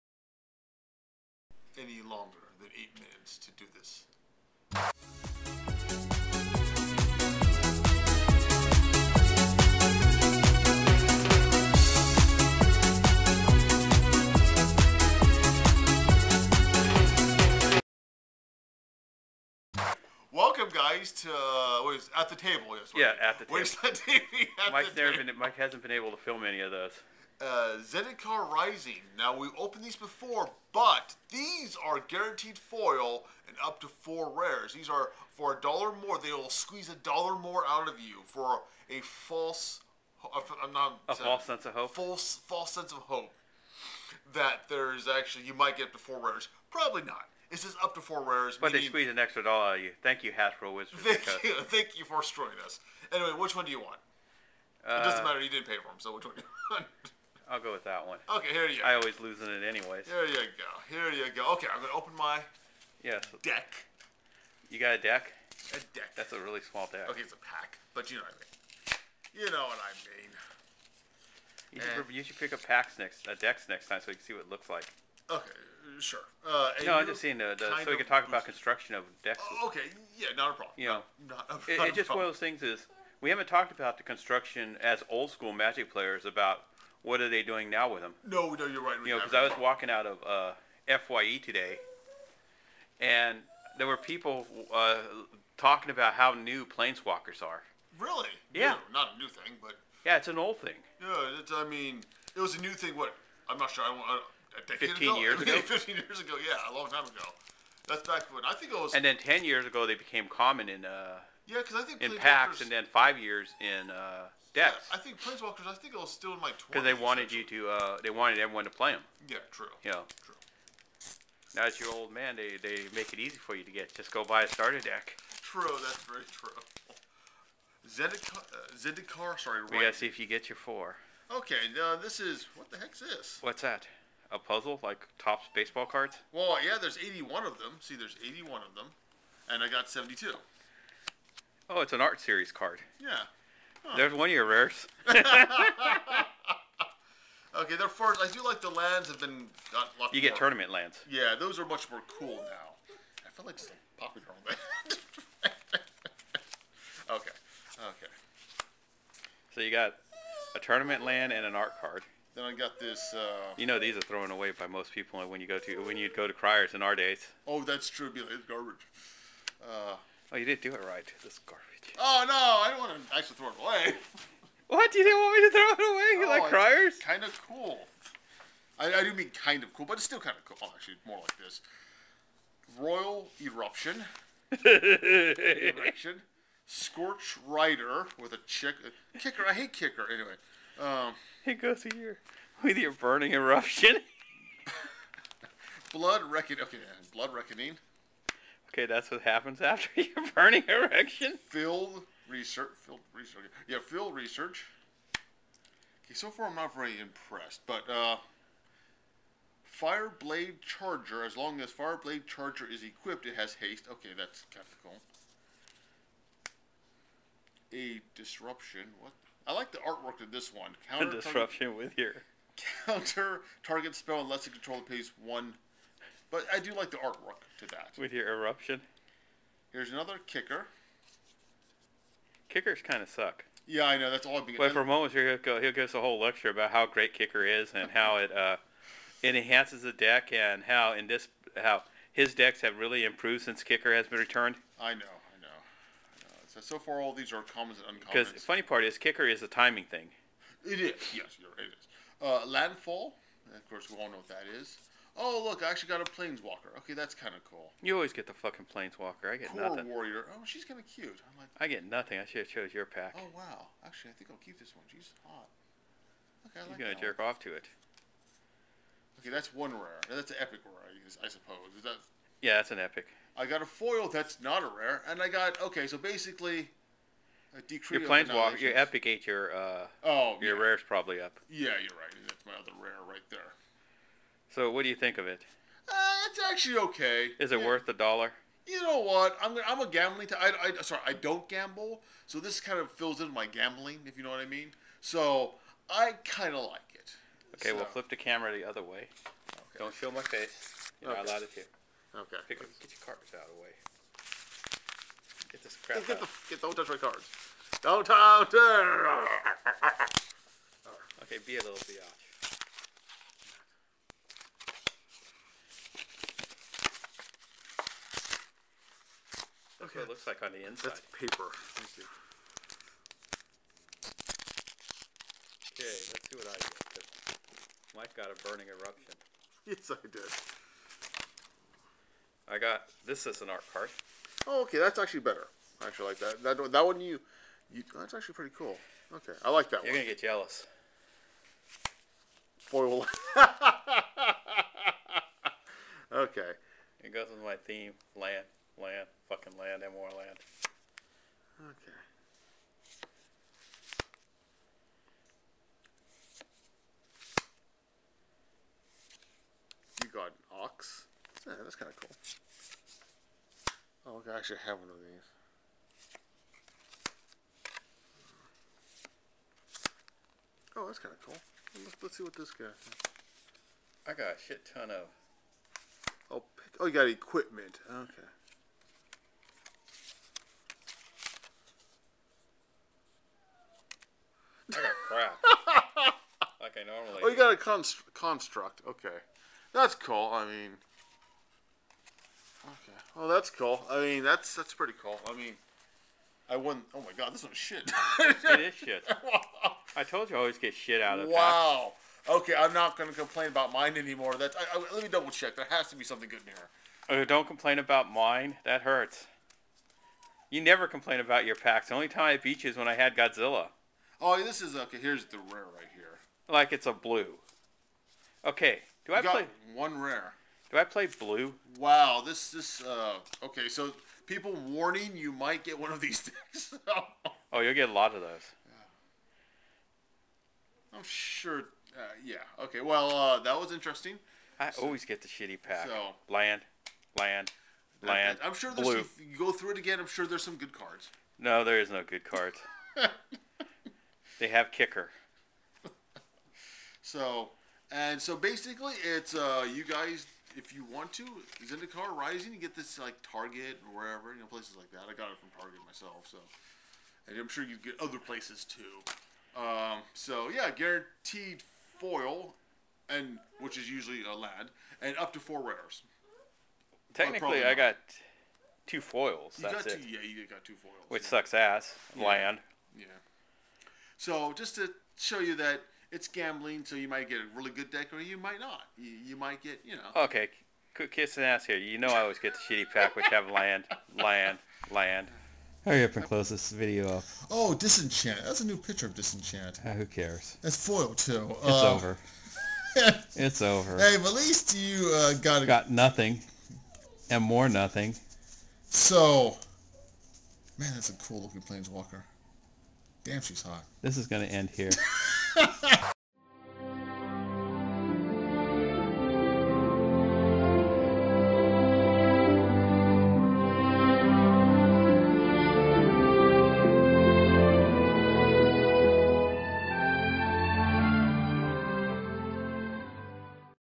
At The Table 2020: Return to Pack Wars
Watch Agis 261 and Monster Mike have pack wars with the newest set of Magic The Gathering called Zendikar Rising.